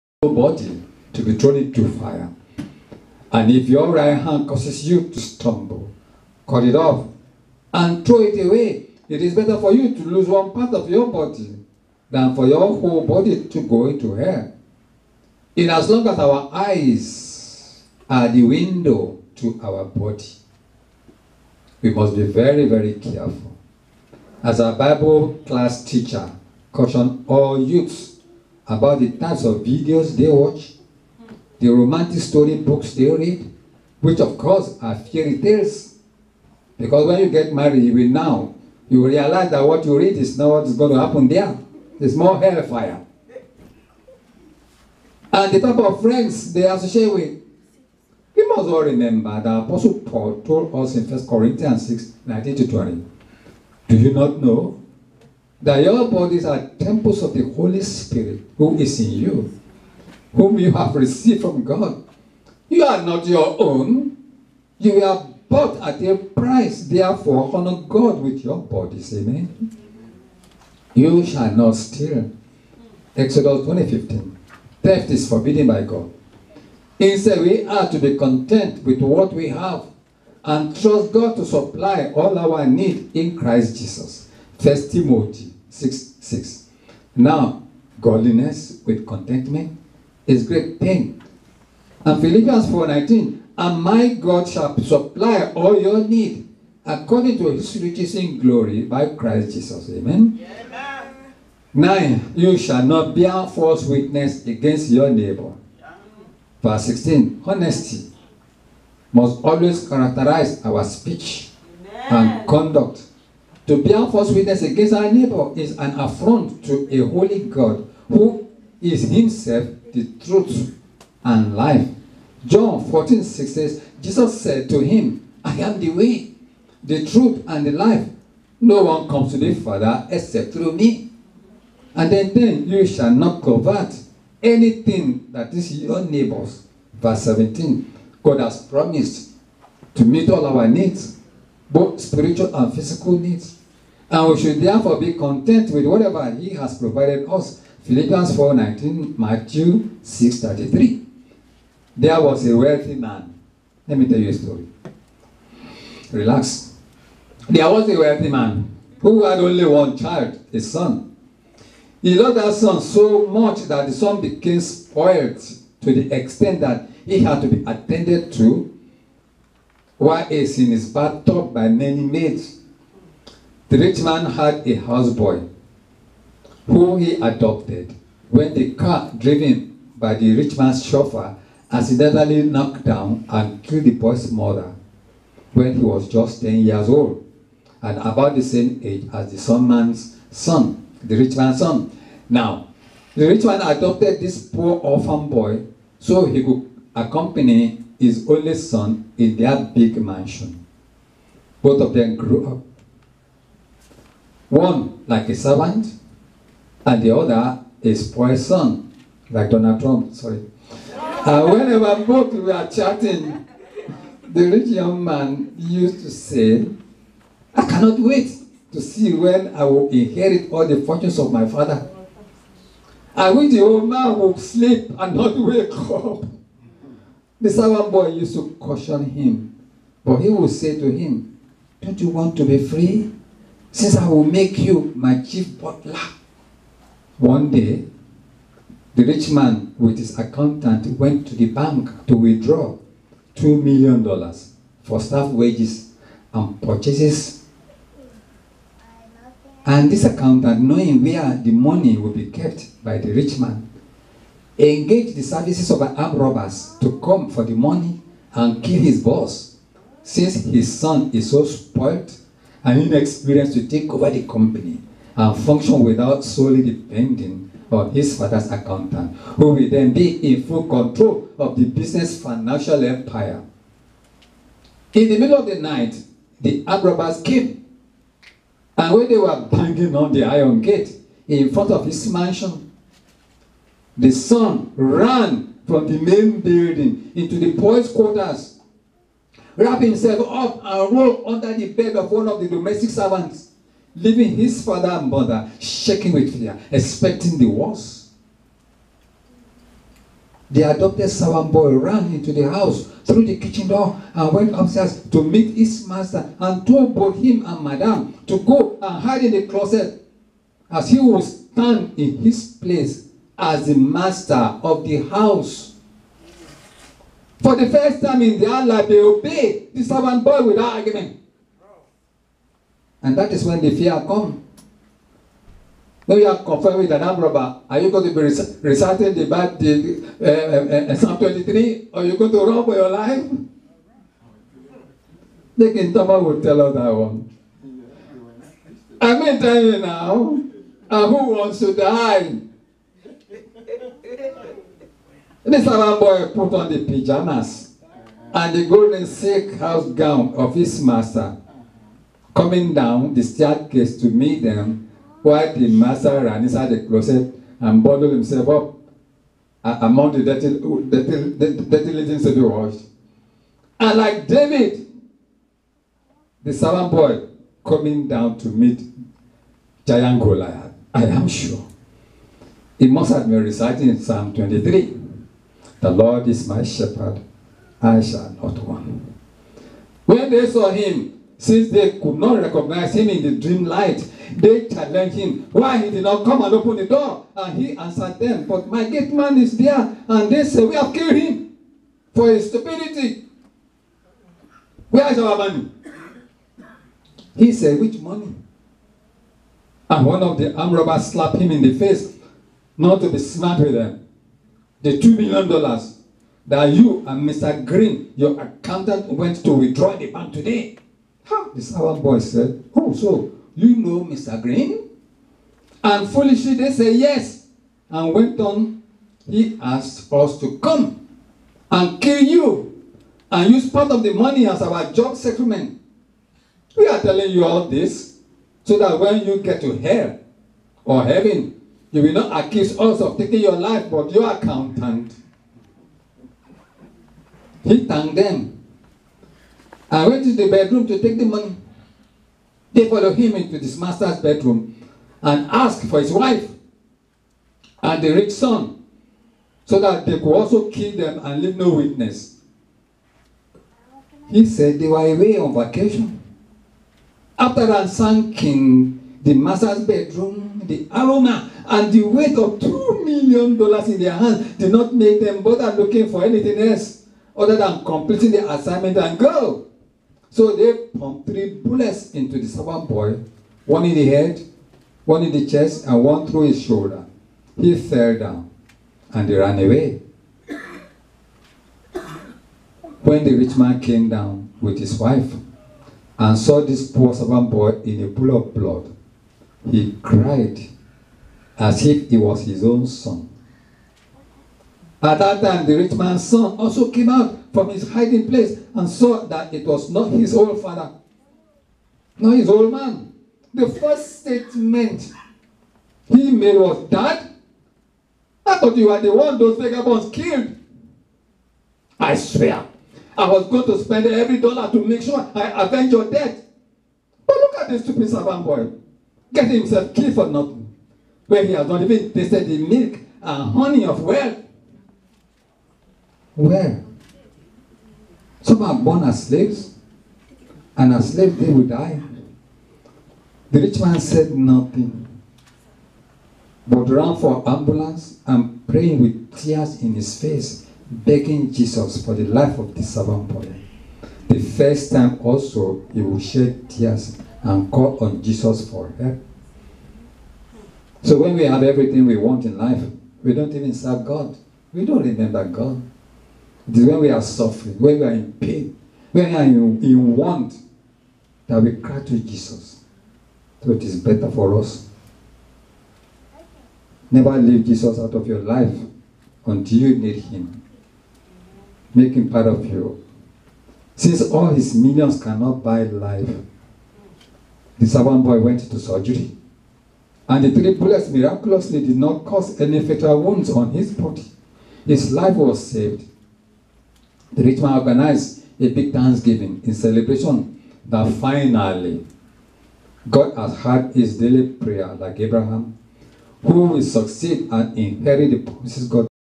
The Power of Love 3
The Ten Commandments:
1. “You shall have no other gods before Me."
-Exodus 20:3 - In view of His sovereignty and goodness (vs. 2), we are commanded to have no other gods before Jehovah.

2. "You shall not make for yourself a carved image..."
-Exodus 20:4 - The first and second commandments go hand in hand, both emphasizing the need to give our worship exclusively to the one true God. The Israelites violated this commandment and crafted their own graven image, a golden calf, even before Moses came down from the Mountain.

3 "You shall not take the name of the Lord your God in vain..."
-Exodus 20:7 - To take God’s name in vain is to use it in an empty or meaningless way. Because “there is no other name under heaven given among men by which we must be saved,” we must be careful to instead treat the name of God with honor and reverence (Acts 4:12).
4."Remember the Sabbath day, to keep it holy."
-Exodus 20:8 - In wisdom and grace, God commanded His people to observe a weekly day of rest. The Sabbath was to be observed in imitation of God’s own work in Creation (Exod. 20:11), as well as His redemptive work (Deut. 5:15).

5."Honor your father and your mother..."
-Exodus 20:12 - The fifth commandment is reiterated by the apostle Paul who describes it as “the first commandment with promise” (Eph. 6:2). Honoring God means honoring the authority He has placed in our lives and what is the promise if we honor not only our biological fathers but all those old enough to be our fathers or mothers; vs 3 says “that it may be well with you and you may live long on the earth.”

6."You shall not murder."
-Exodus 20:13 - The taking of human life is expressly forbidden. Jesus elaborates on this commandment in the Sermon on the Mount, revealing that the sin of murder begins in the heart (Matt. 5:21-22). “You have heard that it was said to those of old, ‘You shall not murder, and whoever murders will be in danger of the judgment.’  But I say to you that whoever is angry with his brother without a cause shall be in danger of the judgment. And whoever says to his brother, ‘Raca!’ shall be in danger of the council. But whoever says, ‘You fool!’ shall be in danger of hell fire.”

7."You shall not commit adultery."
-Exodus 20:14 - The sanctity of marriage is given noteworthy attention throughout Scripture. Christ reminds us in Matthew 5:27-30 that the marriage relationship must be guarded diligently.

8."You shall not steal."
-Exodus 20:15 - Theft is forbidden by God. Instead, we are to be content with what we have and trust God to supply all our need in Christ Jesus (1 Tim. 6:6 – “Now godliness with contentment is great gain. Phil. 4:19 – “And my God shall supply all your need according to His riches in glory by Christ Jesus.

9."You shall not bear false witness against your neighbor."
-Exodus 20:16 - Honesty must always characterize our speech and conduct. To bear false witness against our neighbor is an affront to a holy God Who is Himself "the truth and the life" (John 14:6) - Jesus said to him, “I am the way, the truth, and the life. No one comes to the Father except through Me.

10."You shall not covet...anything that is your neighbor’s."
-Exodus 20:17 - God has promised to meet all our needs, both spiritual and physical, and we should therefore be content with whatever He has provided (Phil. 4:19; Matt. 6:33; Heb. 13:5)